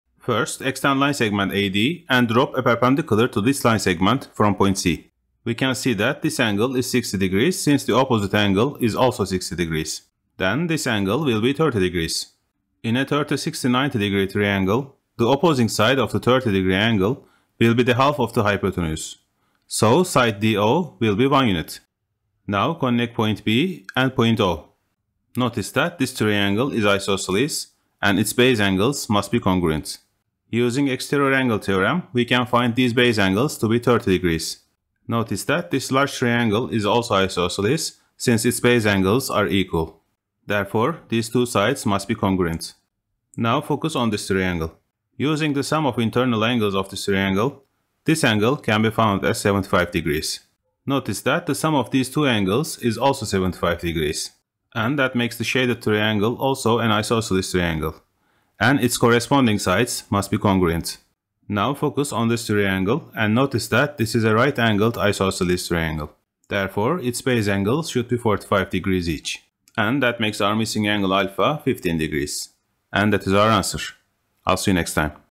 Can you figure this out? (Geometric Approach)
A triangle geometry question which can be hard to solve. We are give 2 angles of 45 degree and 60 degree. Also, we know the lengths of two line segments of the triangle. And we need to find the missing angle α.

Solution of the problem requires the use of;
- Properties of isosceles triangles,
- Properties of opposite angles,
- Properties of 30-60-90 degree triangles,